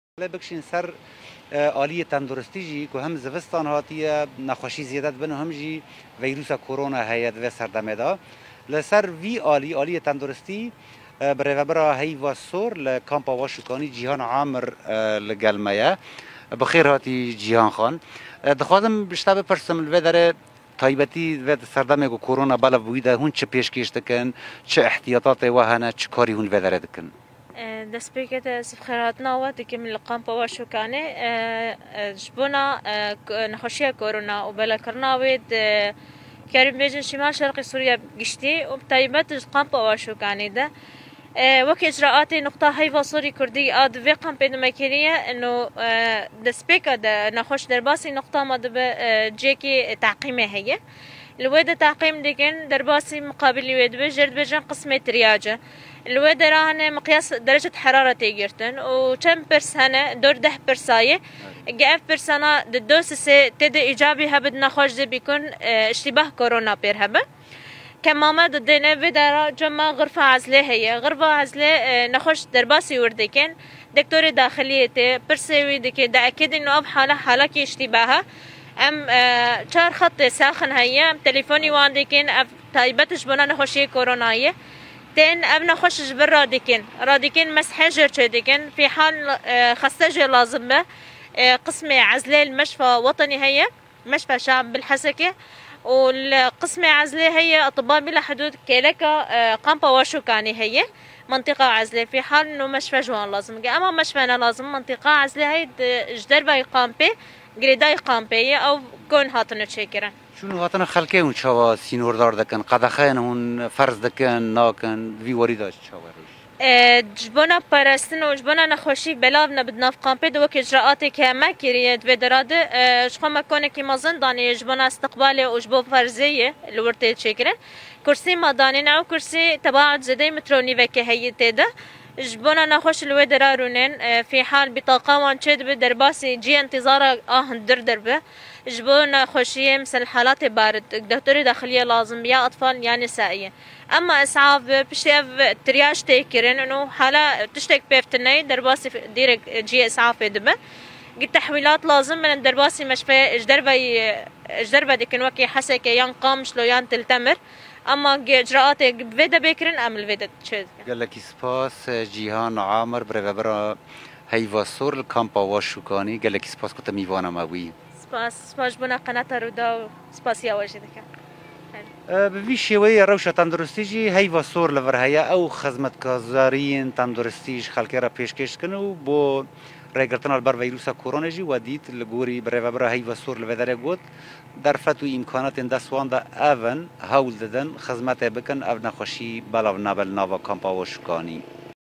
Li ser xweparastina ji Corona li kampa WaşûKanî
Rûdaw li Heyva Sora Kurd
الوقاية من فيروس كورونا في مخيم واشوكاني في الحسكة